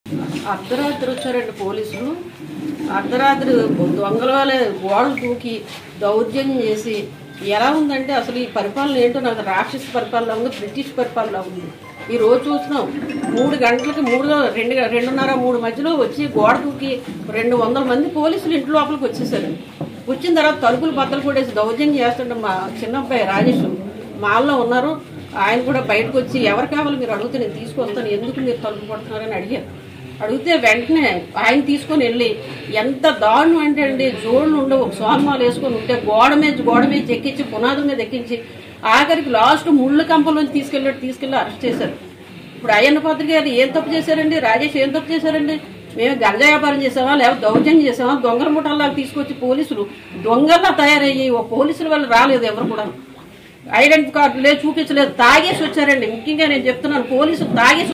TDP Leader Ayyanna Patrudu Wife Shocking Comments on Jagan Govt
Watch► TDP Leader Ayyanna Patrudu Wife Shocking Comments on Jagan Govt